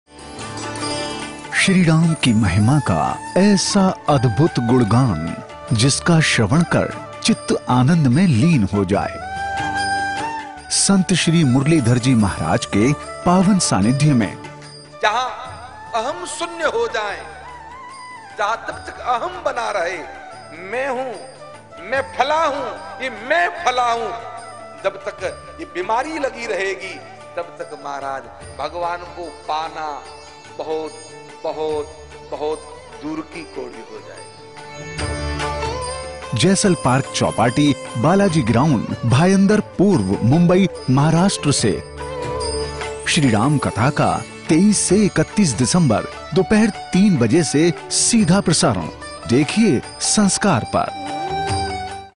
Shri Ramakatha 23 To 31 Dec. 2019 Bhayandar East , Mumbai
संत श्री मुरलीधर जी महाराज जी के मुखारविंद से  "श्री रामकथा"  का भव्य आयोजन होने जा रहा है ,जिसमे आप सभी सादर आमंत्रित है !

कथा स्थल :- जैसल पार्क चौपाटी (बालाजी ग्राउंड) भायंदर (पूर्व) ,महाराष्ट्र

कथा दिनांक  :- 23 से 31 दिसम्बर 2019
समय :  दोप. 3 बजे से 7:00 बजे तक
इस कथा का सीधा प्रसारण संस्कारTV ओर Youtube { } पर होगा !

कथा की हर जानकारी अब आप FACEBOOK से प्राप्त कर सकते है . .

यह कथा आप यु ट्यूब पर LIVE भी सुन सकते है